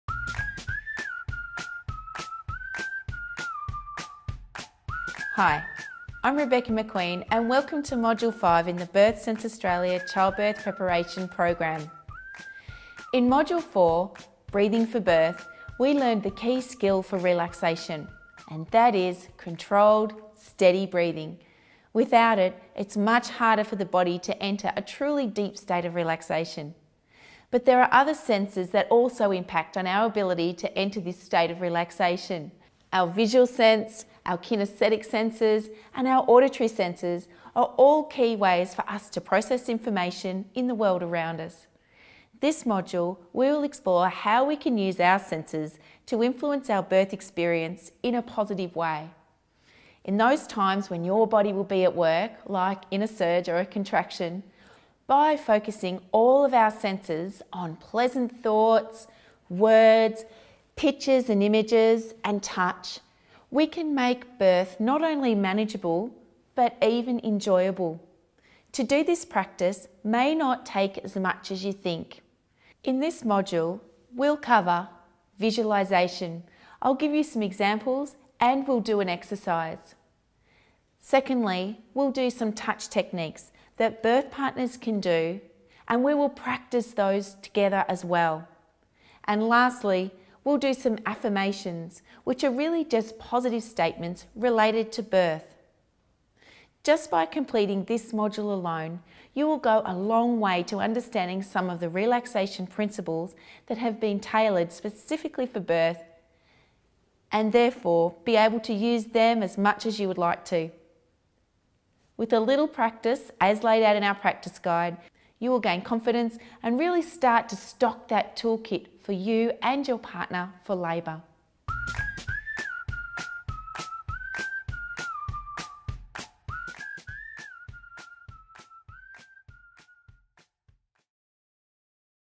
5. Relaxation for Birth Preview
Master your senses for birth using relaxation and mindfulness skills. Our senses control our experience – learn specific visualisation, auditory and kinaesthetic techniques for labour and birth.